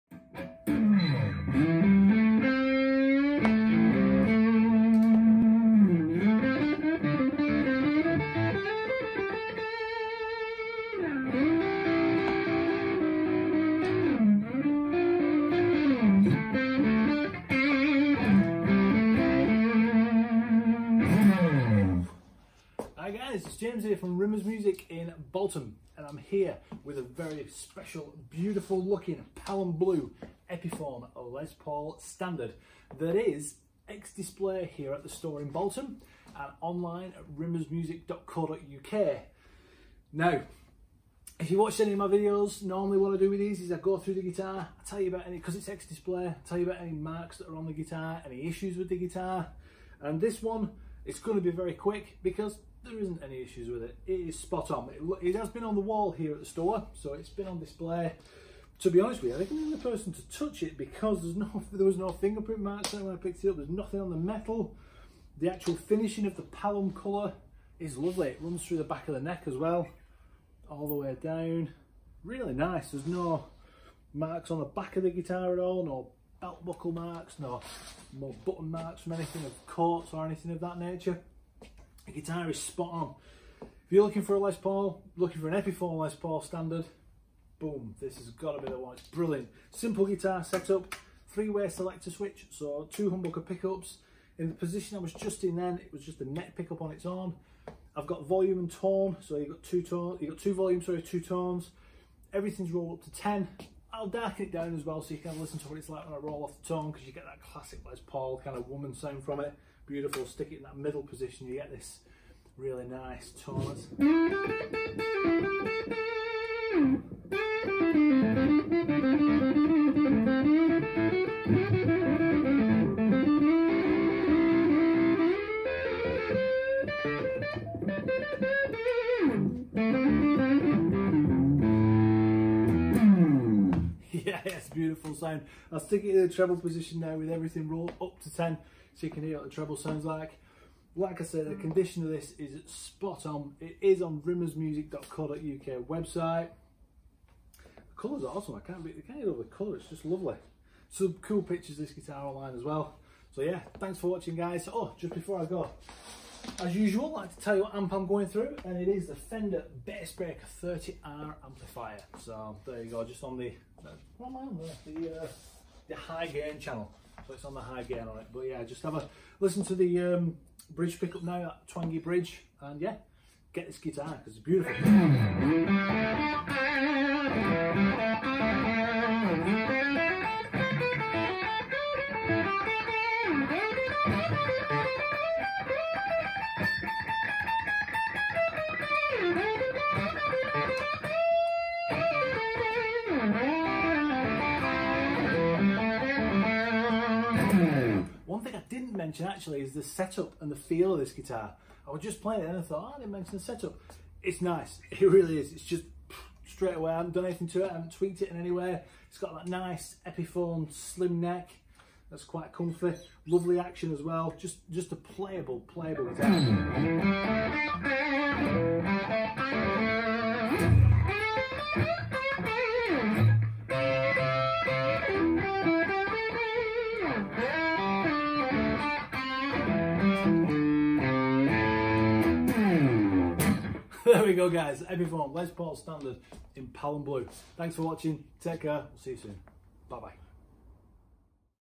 Epiphone Les Paul Standard Pelham Blue | Ex-Display | Bolton Store
Here we have James from Rimmers Music giving you a demonstration of an Epiphone Les Paul Standard in Pelham Blue | Ex-Display at our Bolton store. James is playing through a Fender Bassbreaker 30r amp on the hi-gain channel!
Please find below links to both of these amazing products on the Rimmers Music website.